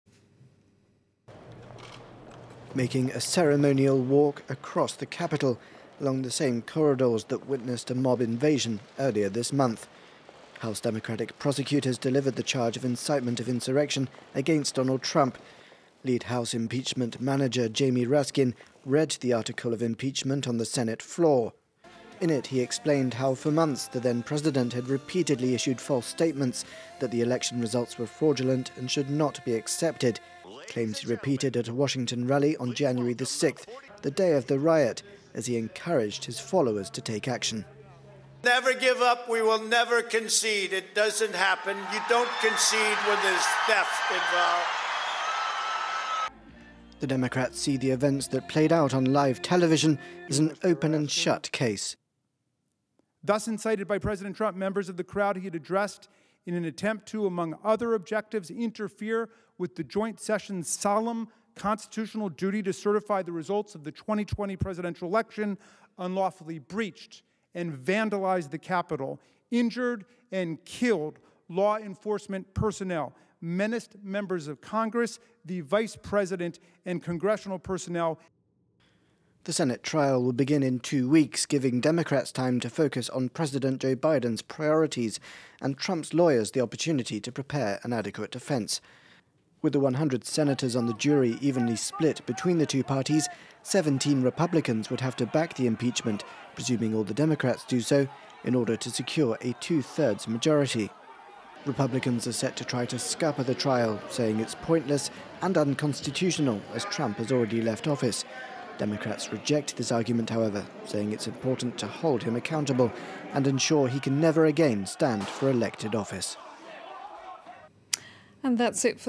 US Republicans scale down Trump criticism as House delivers second impeachment articles to Senate
Democrats from the United States’ House of Representatives have delivered their Impeachment case against Donald Trump to the Senate. But Republican senators, who voiced their anger over the former president’s stance after his supporters stormed the Capitol on January 6, seem to be calming their criticism of Donald Trump and scaling back the calls to convict him over the deadly siege in Washington.